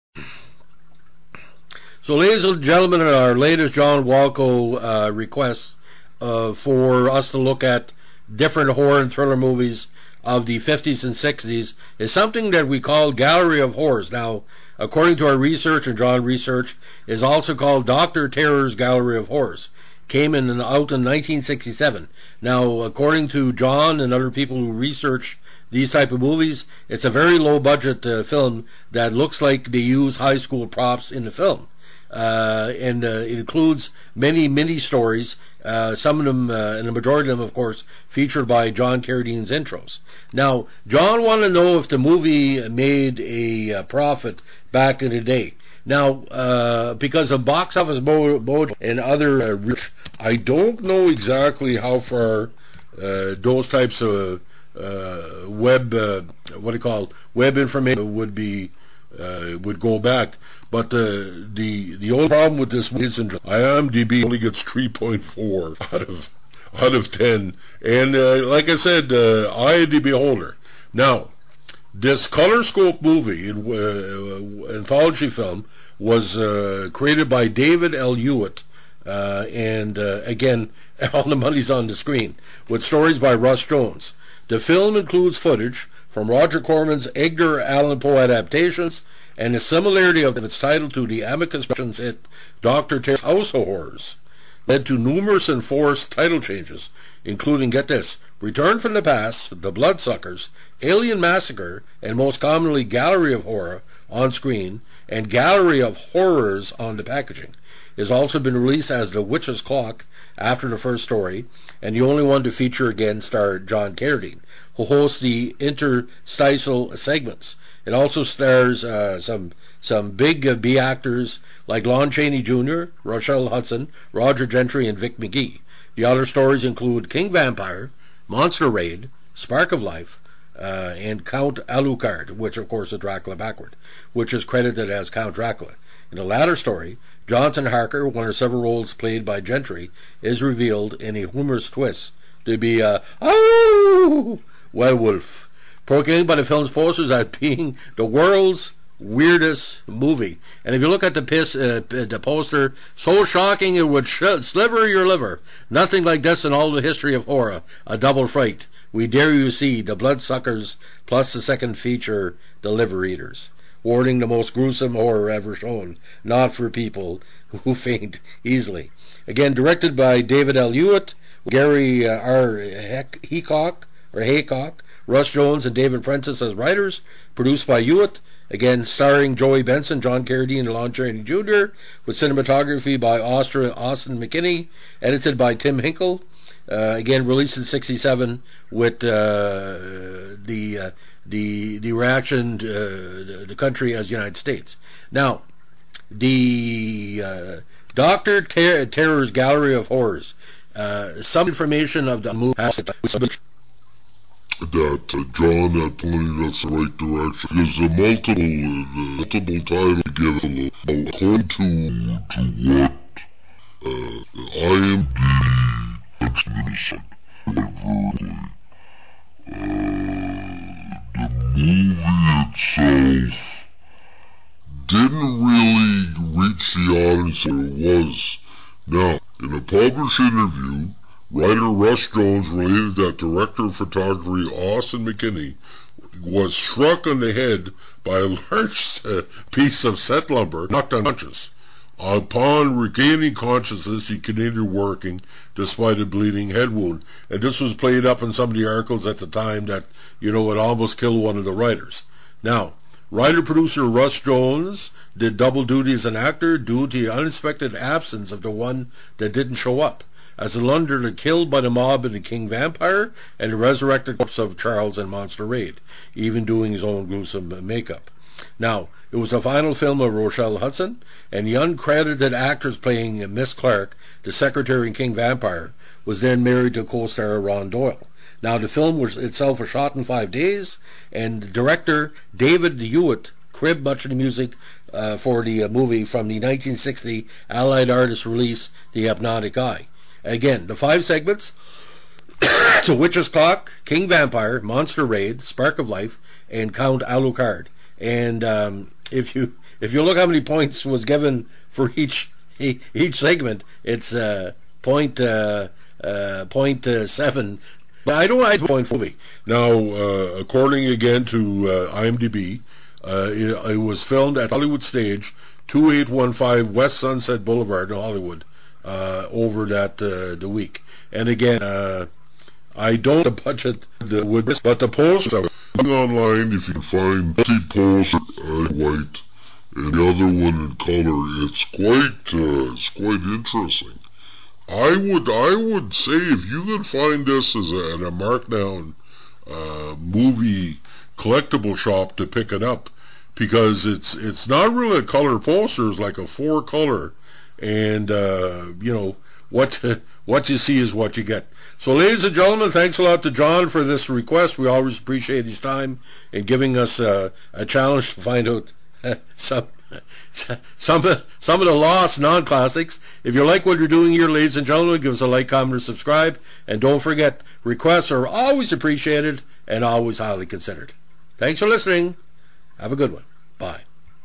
Dr. Terror's Gallery of Horror (1967) Was A Multi-Chapter Cult Movie Draw
John Carradine and Lon Chaney Jr. were the key thespians in this macabre film